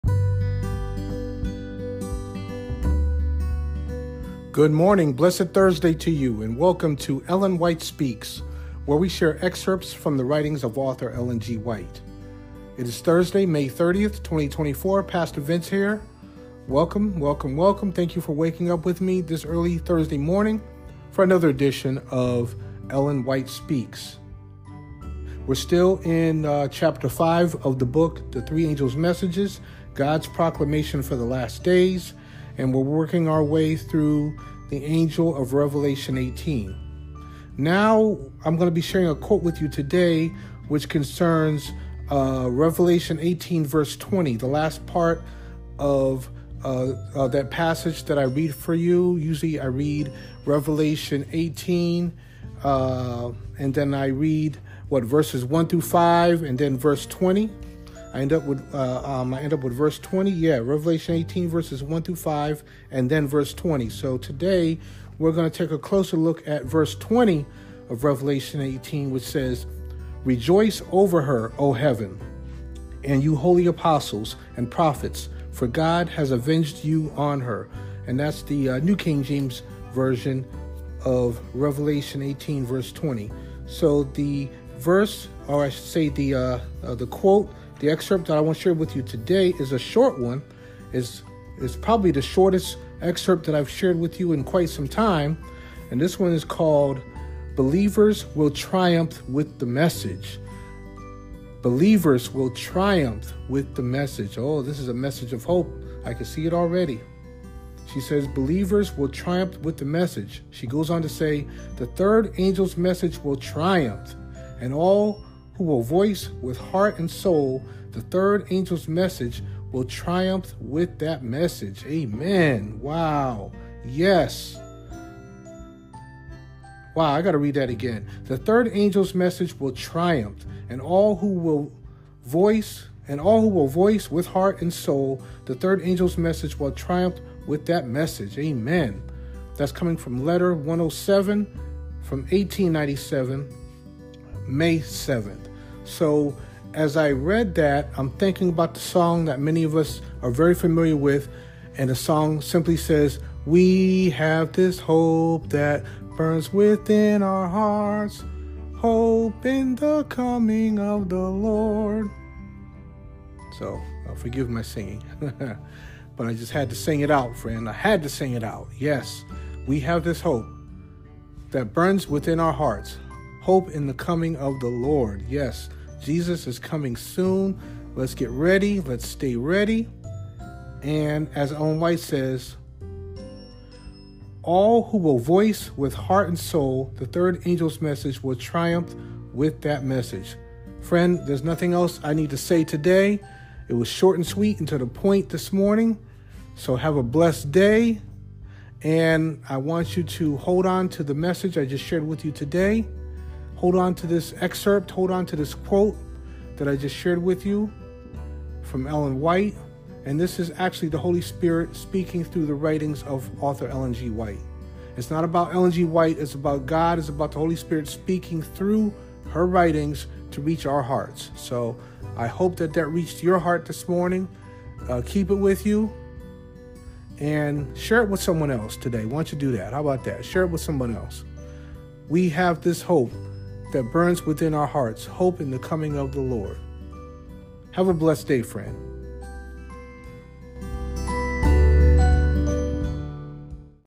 ELLEN WHITE SPEAKS… || “Believers Will Triumph” | May 30, 2024
From Ch. 5 of the book “The Three Angels’ Messages”